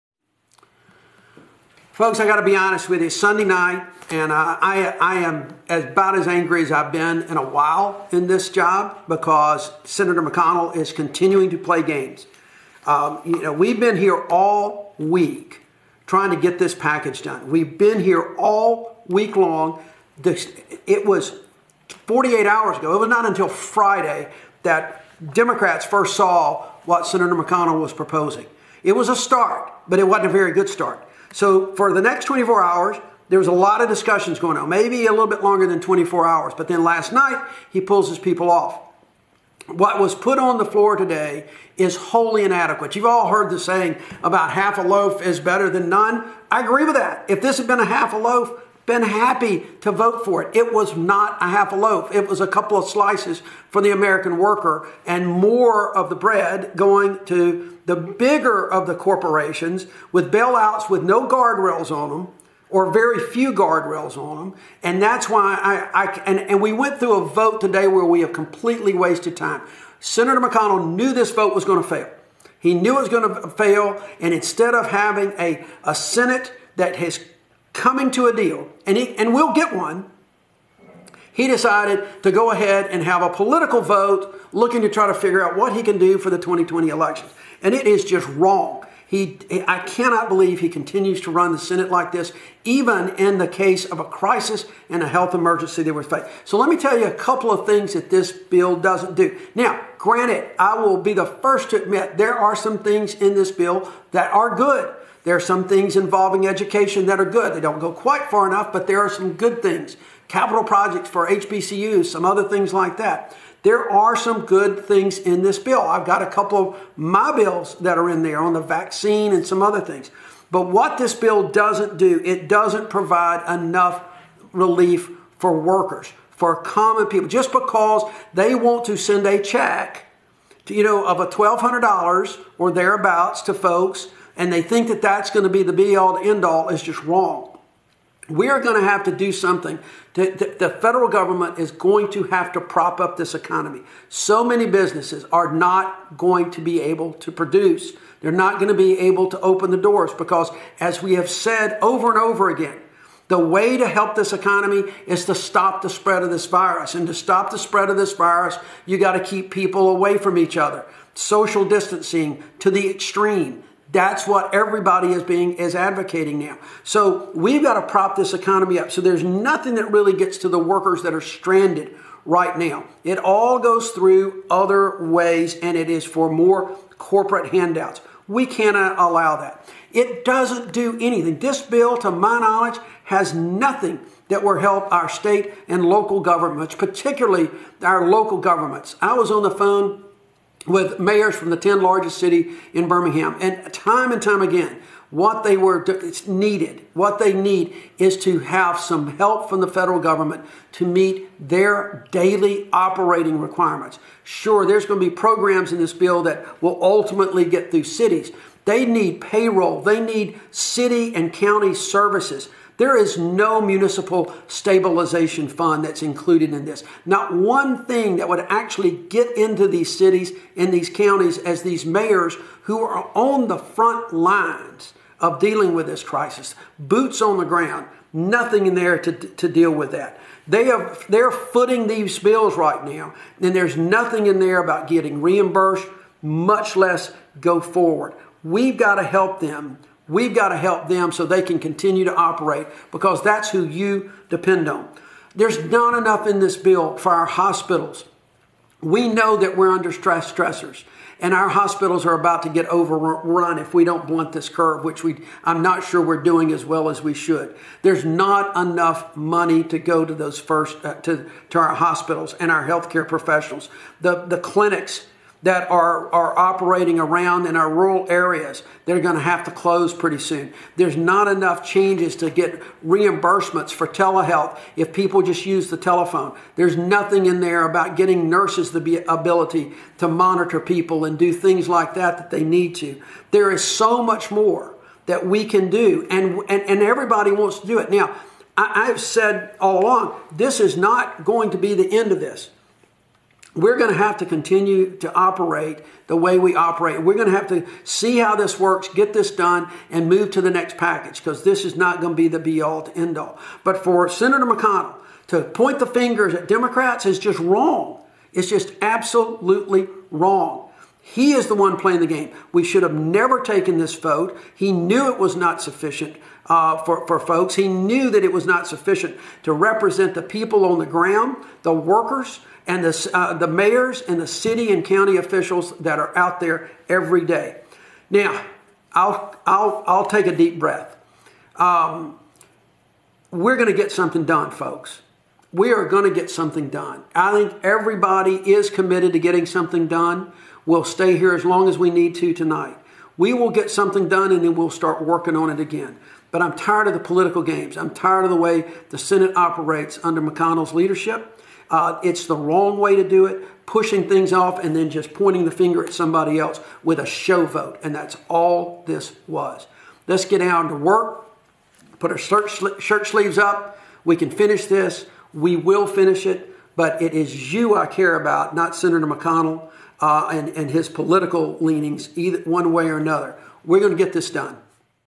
Democratic Senator Doug Jones on why he voted against COVID-19 relief package
U.S. Senator Doug Jones, a Democrat from Alabama, released a video message about why he voted against a procedural motion to advance the third installment of a coronavirus response and relief package.